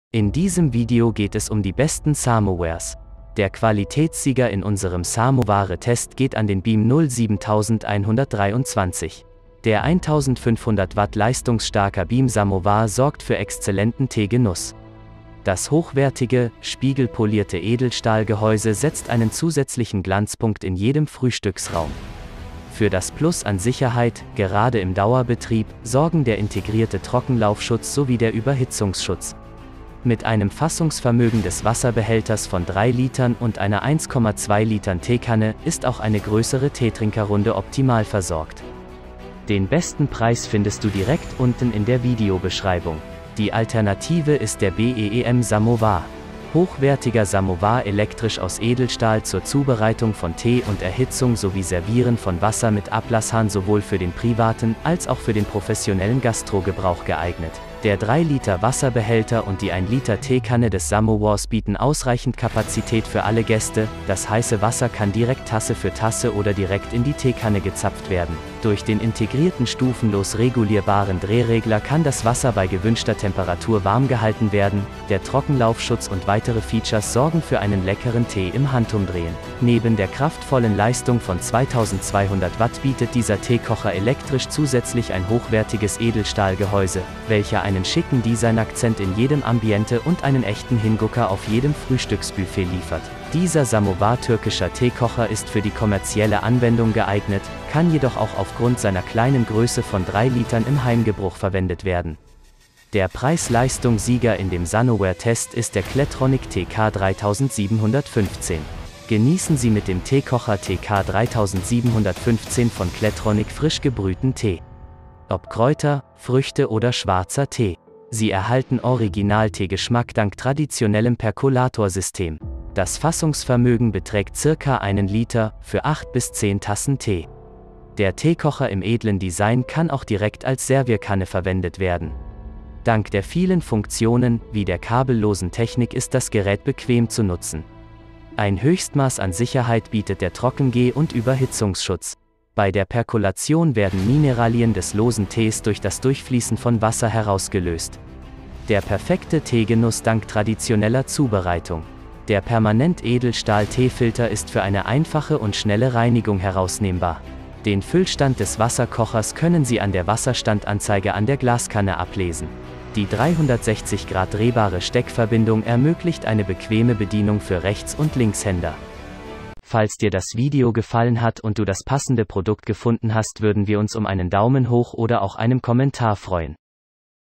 ➤ Samoware Test 2023 | Die besten 3 Samowares
Der Qualitätssieger in unserem Samoware Test geht an den Beem 0 7123.

Der 1500 Watt leistungsstarker Beem Samowar sorgt für exzellenten Tee Genuss. Das hochwertige, spiegelpolierte Edelstahlgehäuse setzt einen zusätzlichen Glanzpunkt in jedem Frühstücksraum. Für das Plus an Sicherheit, gerade im Dauerbetrieb, sorgen der integrierte Trockenlaufschutz sowie der Überhitzungsschutz. Mit einem Fassungsvermögen des Wasserbehälters von 3 Litern und einer 1,2 Liter Teekanne, ist auch eine größere Teetrinkerrunde optimal versorgt.

Die alternative ist der BEEM Samowar.

Hochwertiger Samowar elektrisch aus Edelstahl zur Zubereitung von Tee und Erhitzung sowie Servieren von Wasser mit Ablasshahn sowohl für den privaten , als auch für den professionellen Gastro Gebrauch geeignet.
Der 3 Liter Wasserbehälter und die ein Liter Teekanne des Samowars bieten ausreichend Kapazität für alle Gäste, das heiße Wasser kann direkt Tasse für Tasse oder direkt in die Teekanne gezapft werden.
Durch den integrierten stufenlos regulierbaren Drehregler kann das Wasser bei gewünschter Temperatur warm gehalten werden, der Trockenlaufschutz und weitere Features sorgen für einen leckeren Tee im Handumdrehen.
Neben der kraftvollen Leistung von 2200 Watt bietet dieser Teekocher elektrisch zusätzlich ein hochwertiges Edelstahlgehäuse, welcher einen schicken Designakzent in jedem Ambiente und einen echten Hingucker auf jedem Frühstücksbuffet liefert.
Dieser Samowar Türkischer Teekocher ist für die kommerzielle Anwendung geeignet, kann jedoch auch aufgrund seiner kleinen Größe von 3 Liter im Heimgebruch verwendet werden.
Der Preis Leistung Sieger in dem Sanoware Test ist der Clatronic TK 3715.

Genießen Sie mit dem Teekocher TK 3715 von Clatronic frisch gebrühten Tee. Ob Kräuter, Früchte oder Schwarzer Tee. Sie erhalten original Tee Geschmack dank traditionellem Perkolator System. Das Fassungsvermögen beträgt ca einen Liter, für 8 bis 10 Tassen Tee. Der Teekocher im edlen Design kann auch direkt als Servierkanne verwendet werden. Dank der vielen Funktionen, wie der kabellosen Technik ist das Gerät bequem zu nutzen. Ein Höchstmaß an Sicherheit bietet der Trockengeh und Überhitzungsschutz.

Bei der Perkolation werden Mineralien des losen Tees durch das Durchfließen von Wasser herausgelöst. Der perfekte Teegenuss dank traditioneller Zubereitung.

Der permanent Edelstahl Teefilter ist für eine einfache und schnelle Reinigung herausnehmbar.

Den Füllstand des Wasserkochers können Sie an der Wasserstandanzeige an der Glaskanne ablesen.

Die 360 Grad drehbare Steckverbindung ermöglicht eine bequeme Bedienung für Rechts und Linkshänder.